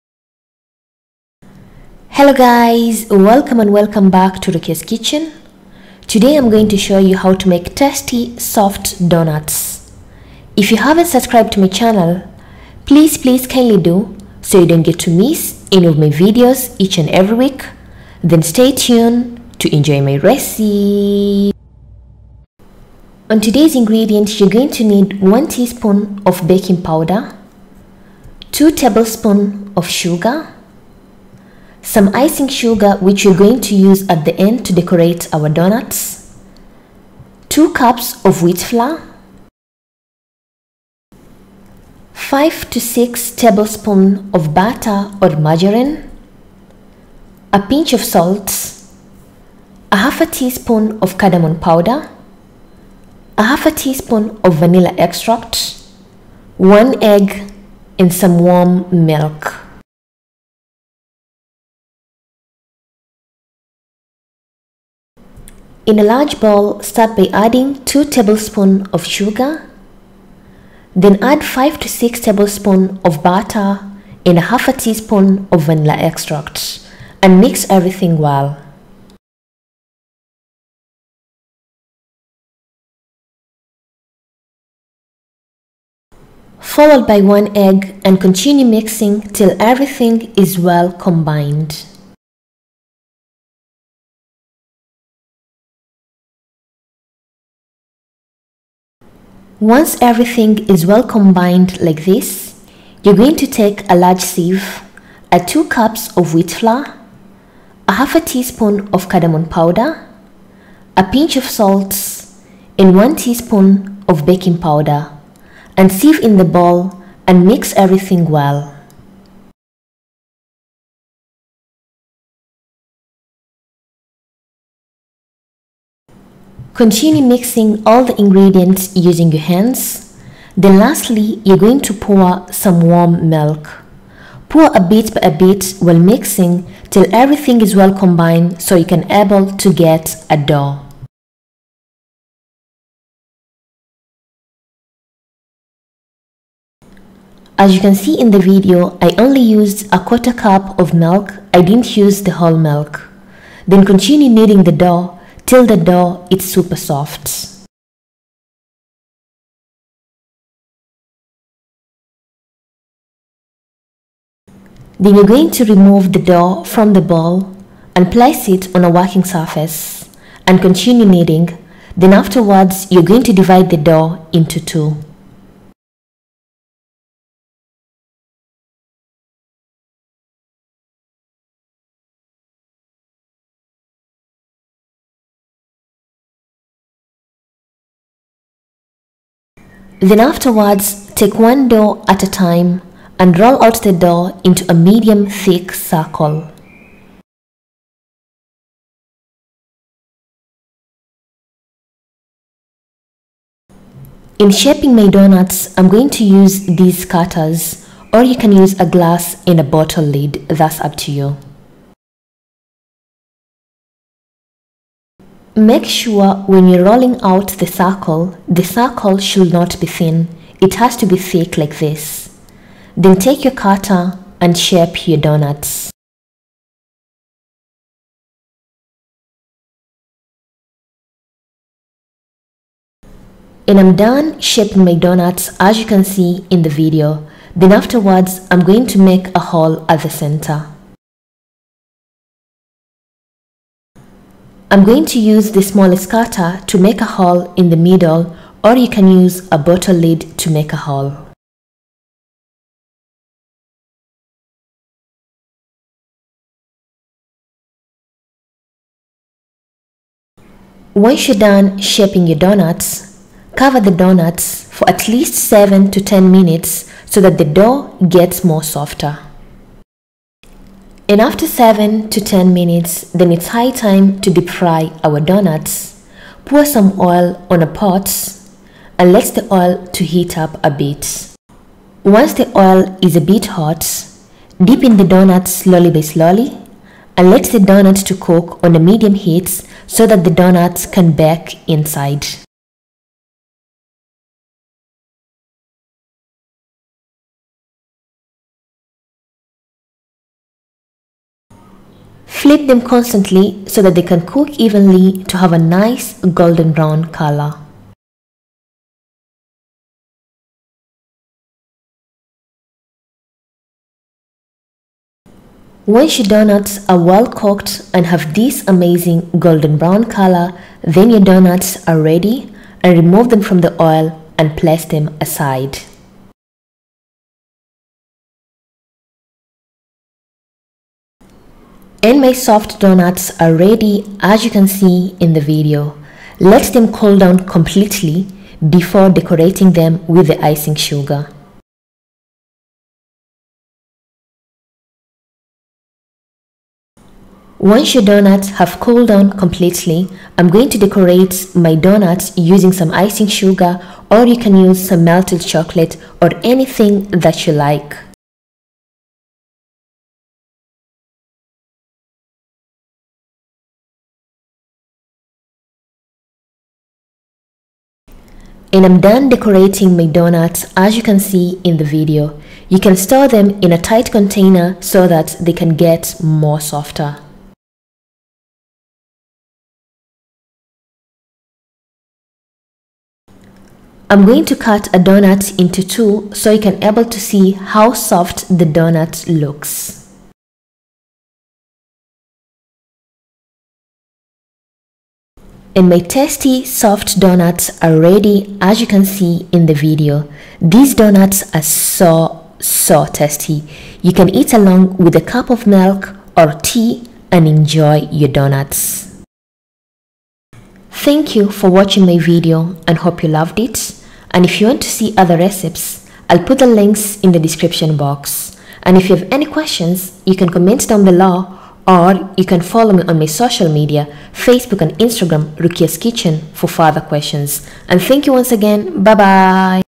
Donuts 🍩 | How to make soft donuts | Donuts recipe | Swahili donuts .
Cookpad: Rukias kitchen

Ingredients:
Wheatflour
Bakingpowder
Egg
Sugar
Butter
Vanila
Milk
Cardamom powder